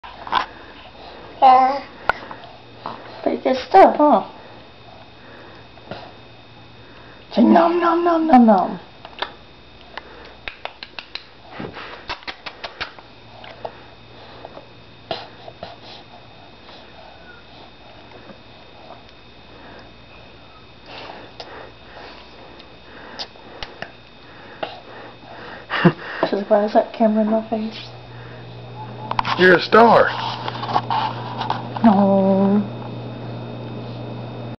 Alyssa growing up=)
Eating mac n cheese with momma and daddy